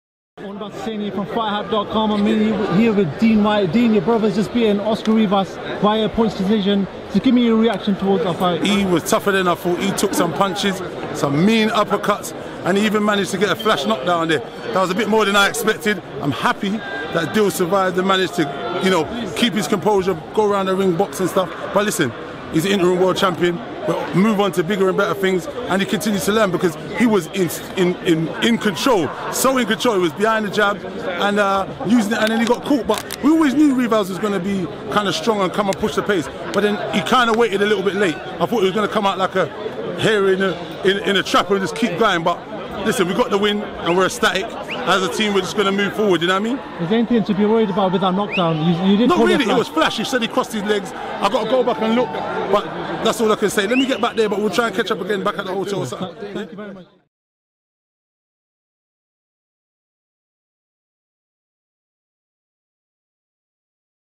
DEAN WHYTE BRUTALLY HONEST ON DILLIAN'S "TOUGHER THAN I EXPECTED" WIN OVER OSCAR RIVAS
FightHype.com was on hand in London, England where newly-crowned interim WBC heavyweight champion Dillian Whyte defeated contender Oscar Rivas via unanimous decision. You don't want to miss what those in attendance had to say about the fight and much more. Check it out!